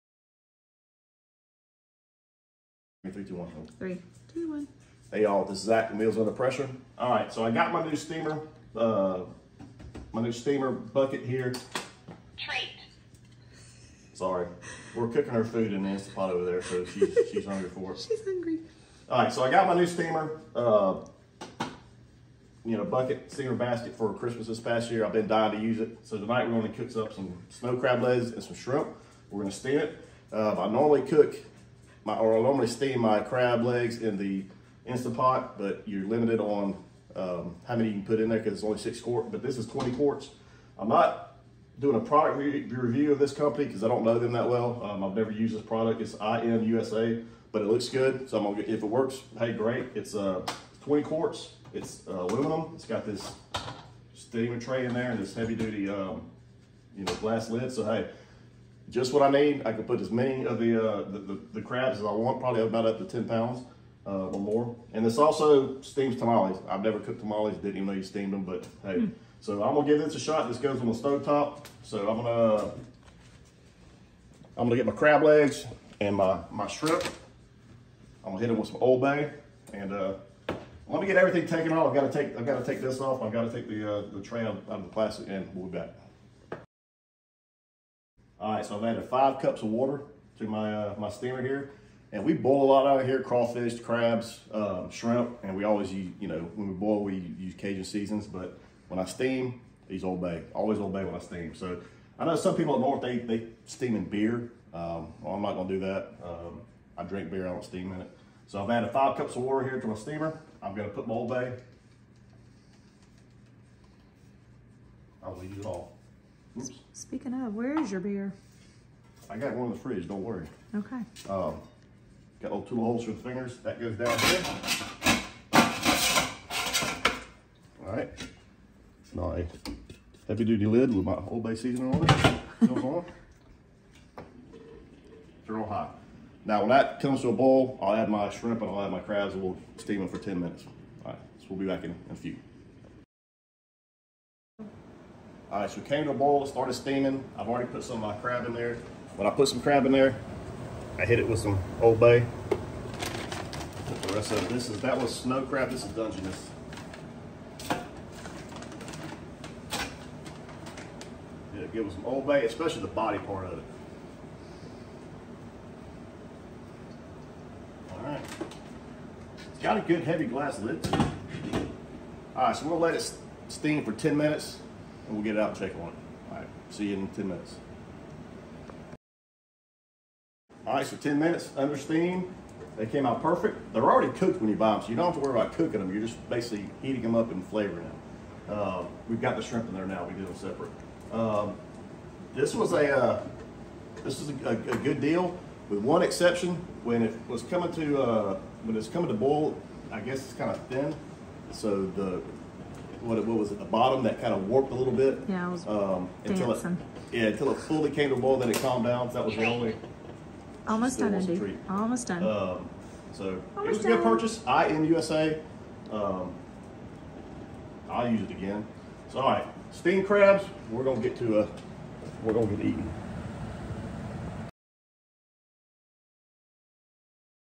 How to Steam Crab Legs | Old Bay Recipe
Steaming is one of the easiest ways to heat up and season crab legs.  We like Old Bay, but you can use whatever seasoning you like.